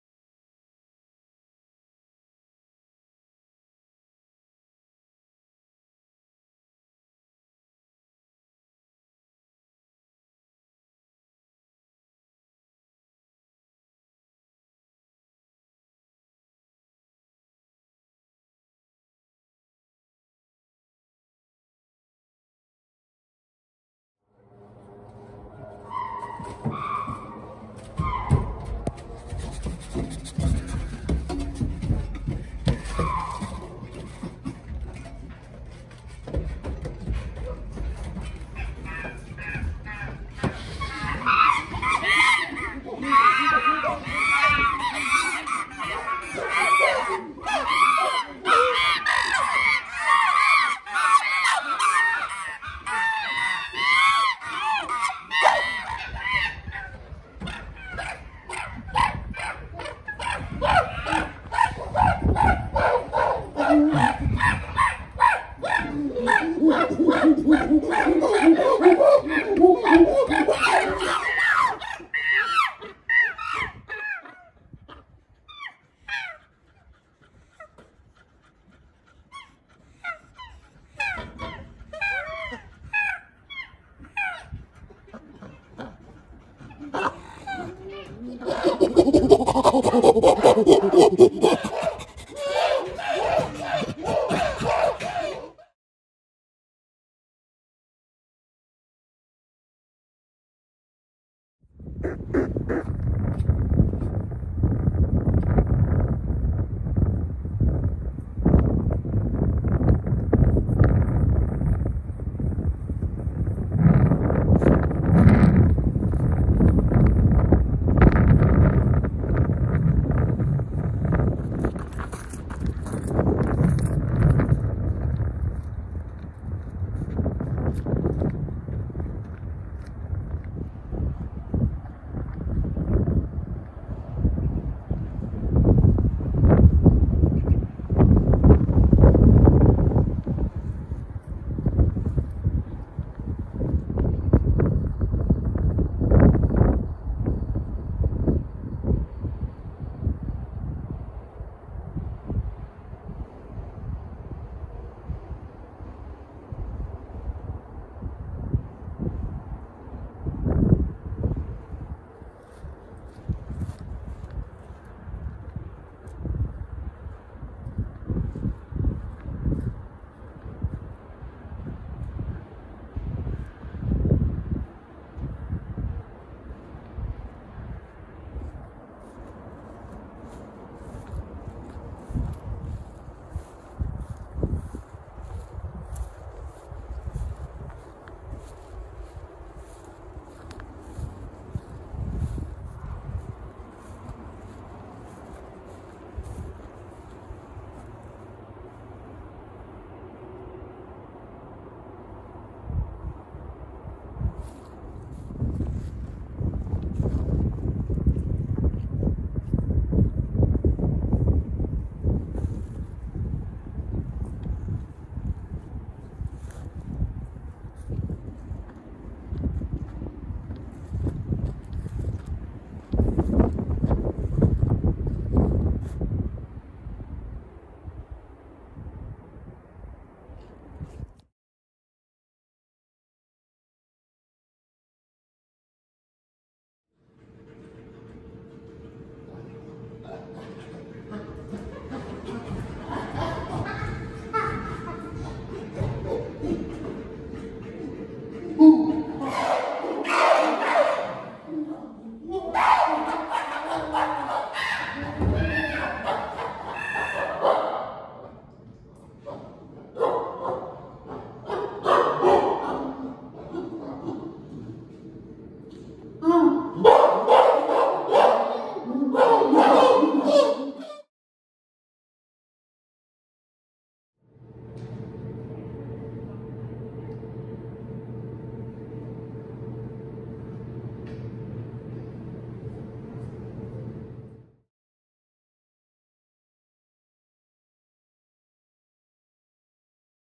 Nosy Neighbors
A new group of rescued chimps explores a short tunnel that will connect to their open-top habitat while the original group watches and then stakes out their territory.